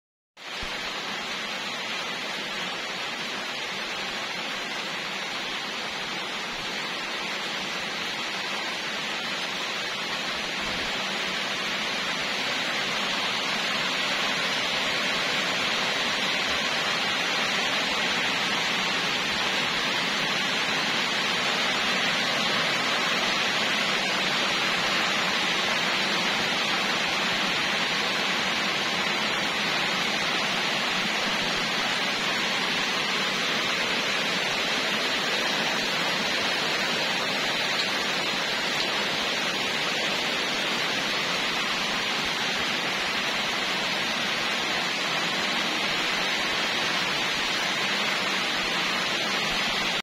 mr potato the cat watching the rain
mr potato the cat watching the rain from the window